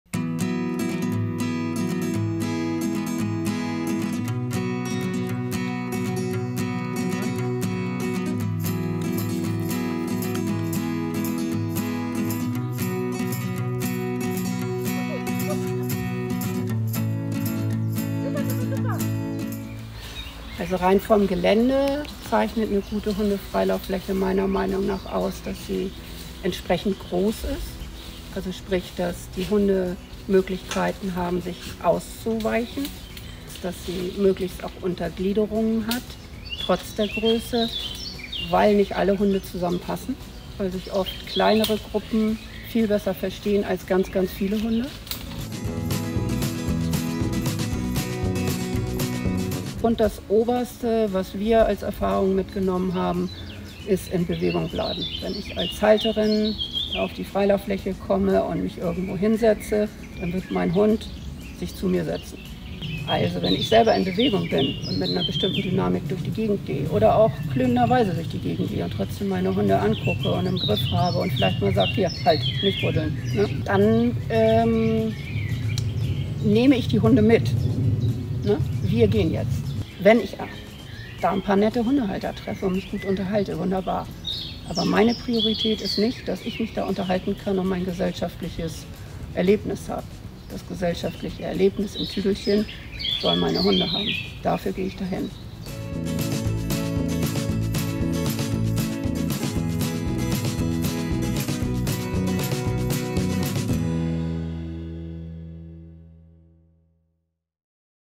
Freilauffläche in Lauenbrück – Spaß für Mensch und Hund
(Erstveröffentlichung: 14. Oktober 2021)

12.000  Quadratmeter umfasst das Gelände am Wirtschaftsweg abgehend vom Benkeloher Weg in Lauenbrück. Die Freilauffläche ist komplett eingezäunt und in verschieden große Bereiche unterteilt. Schleusen und Pforten ermöglichen ein ruhiges Ankommen für die Hunde sowie ein Spielen und Toben auch in Kleingruppen. Die Benutzung steht allen Hundehaltern offen und ist kostenlos. Der Verein freut sich allerdings über Spenden.